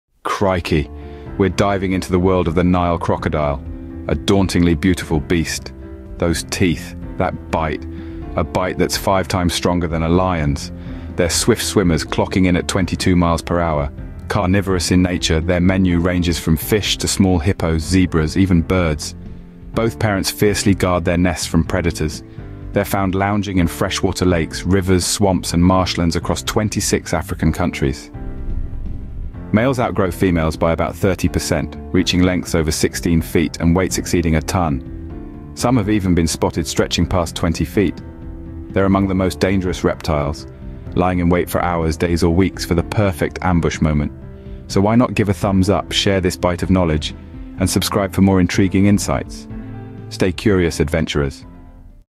The Secret Life of: The Nile Crocodile - A Beautiful Beast
Dive into the heart of Africa with our latest short wildlife documentary, as we explore the fascinating world of the Nile Crocodile! 🐊 From the banks of the mighty Nile River to its strategic hunting techniques, this video offers a captivating glimpse into the life of one of Africa's apex predators.